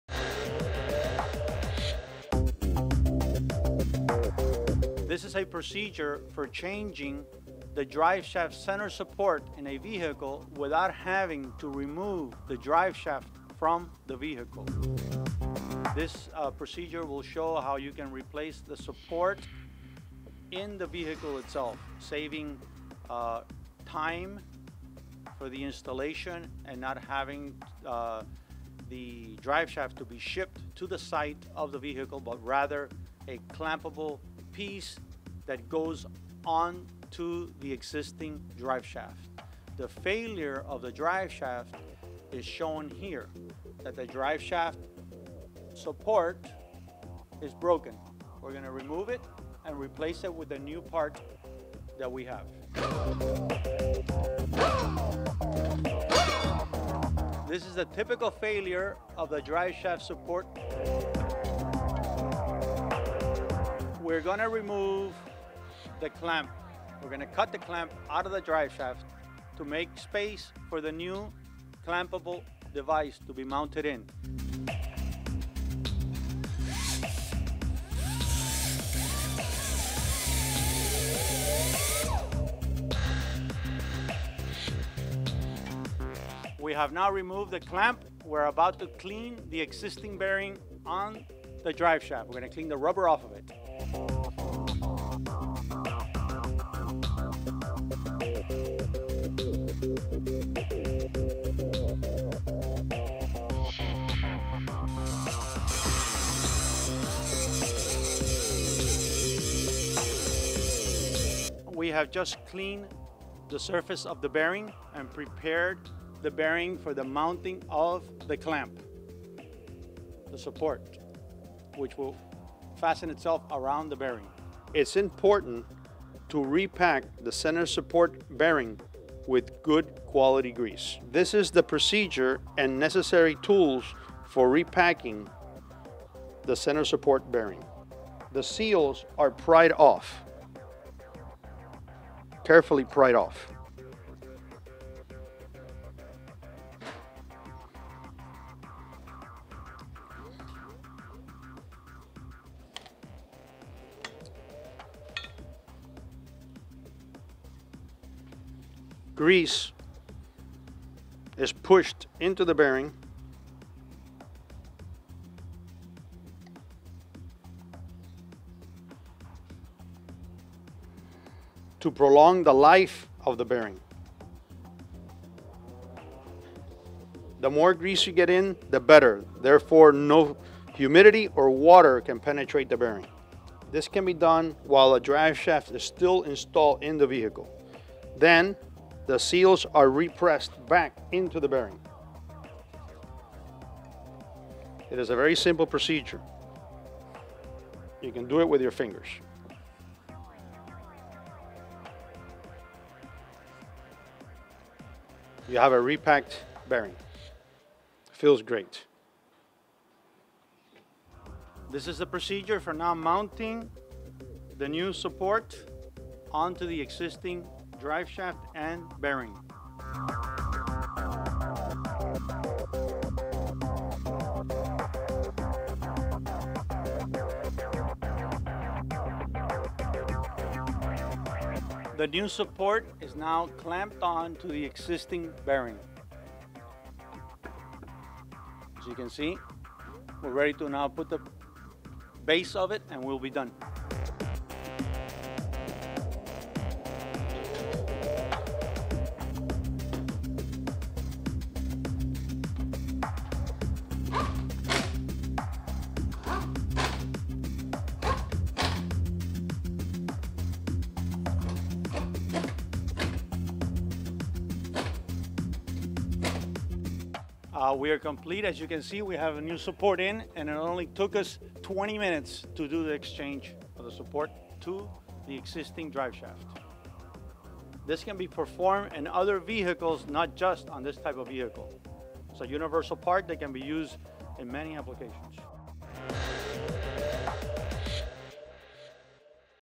Cayenne / VW Touareg Drive Shaft Support Bearing Repair
The innovative Porsche Cayenne Drive Shaft Clamping Center Support (DSCCS) by EPS is the only true Permanent fix to premature bearing support failures. This Patent-Pending device does not require you to remove your Drive Shaft, saving you hundreds in labor & downtime. This end-all solution to failing Porsche Cayenne Bearing Supports. The Vertex upgraded Cayenne Drive Shaft Center Support is the simple choice.